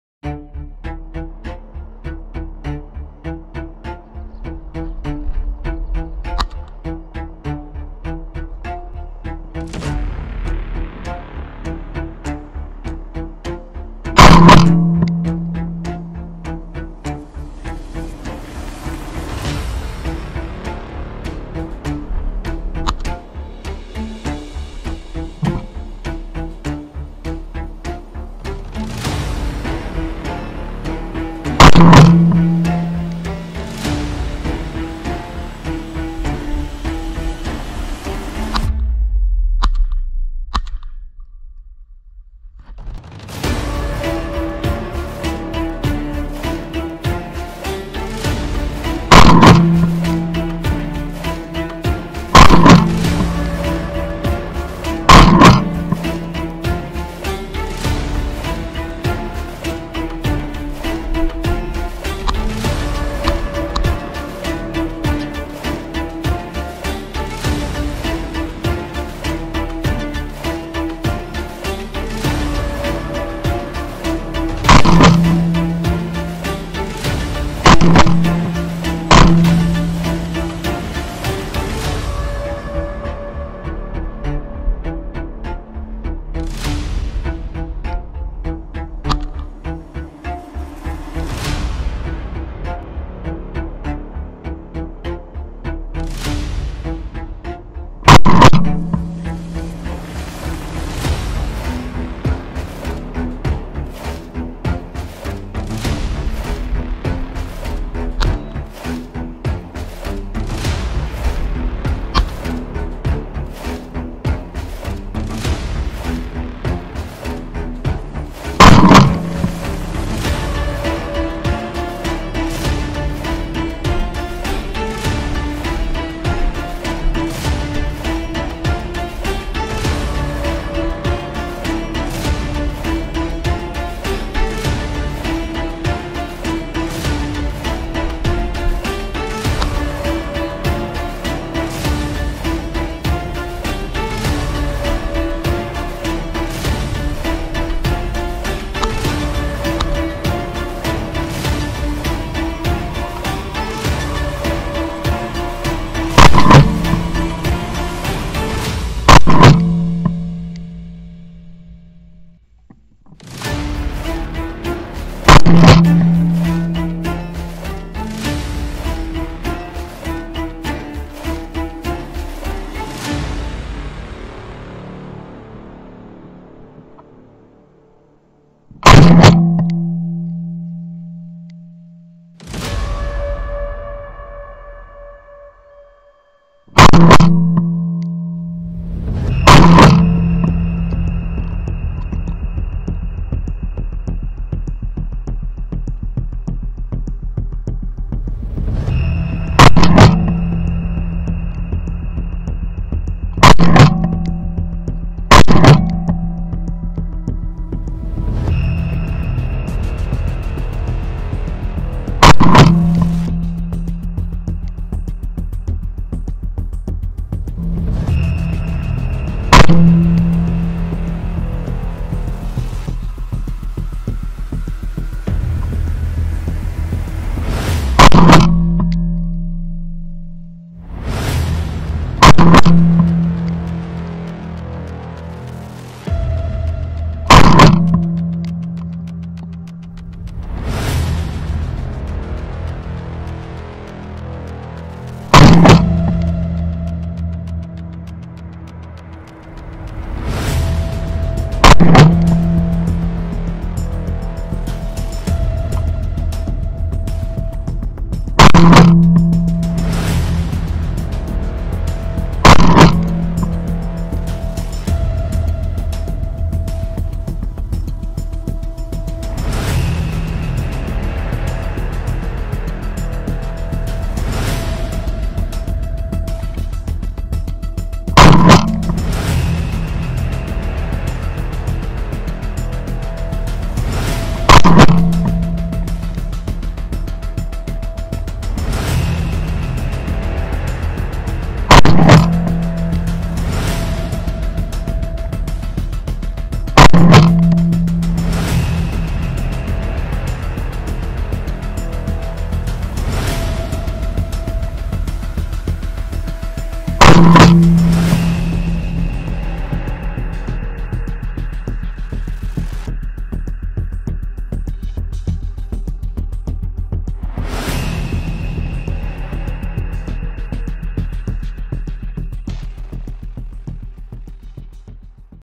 Dove hunt season 2023 using Shotkam shots and Gopro Gemini Chokes with Beretta shotgun
A nice video from the Channels Friend Ralph Aaraj hunting  Doves in Syria